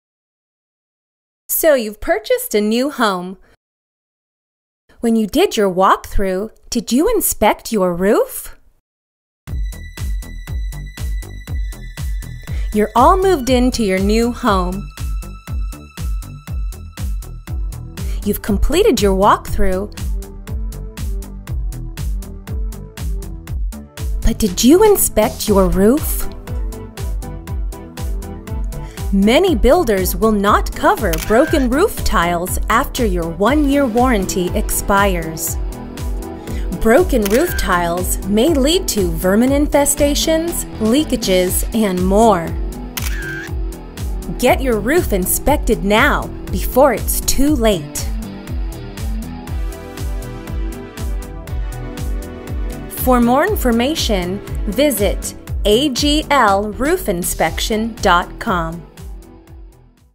AGL Roof Inspection
This happened to a friend that bought a new home and didn't inspect the roof during the Walk-Thru. Don't let it happen to you. get your roof checked before your warranty runs out.